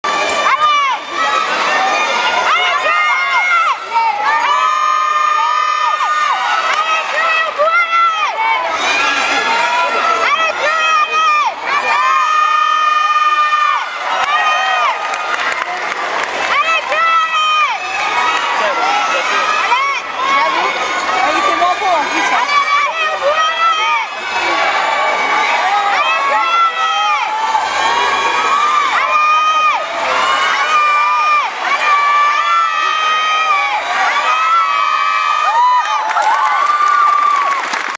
Chloé Stanic Barres Finale Coupe de France Chalons en Champagne 2010
Chloé Stanic (USEAB Avoine Beaumont) aux Barres en Finale de la Coupe de France à Chalons en Champagne le 05/06/2010.